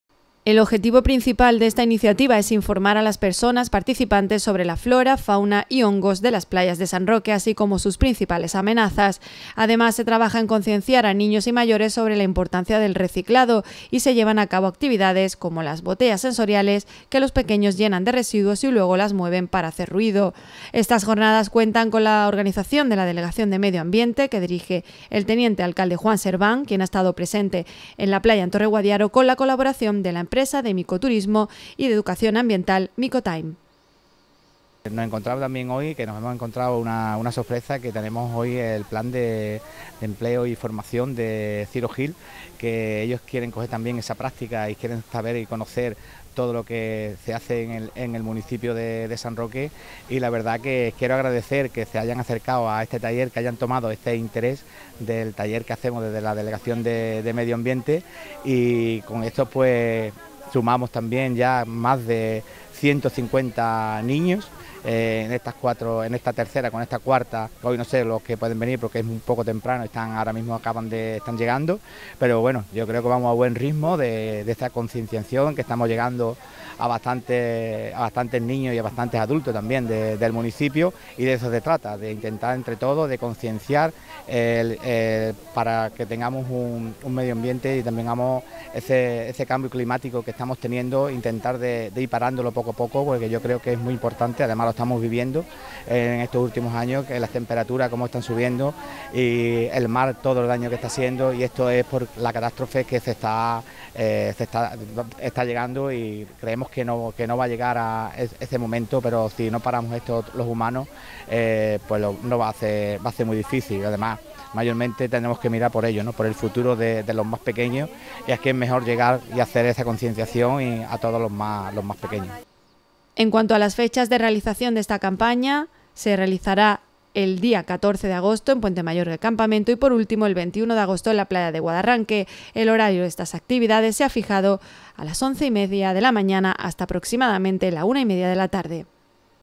NOTICIA RECAPACICLA TORREGUADIARO 7 AGOSTO 2022
La Delegación de Medio Ambiente del Ayuntamiento de San Roque desarrolla cada domingo de los meses de julio y agosto actividades de educación y sensibilización ambiental en el litoral sanroqueño, dirigidas principalmente a menores de edad. Hoy, domingo, han sido en Torreguadiaro ante un día espectacular, climatológicamente hablando, y con numerosos pequeños y mayores.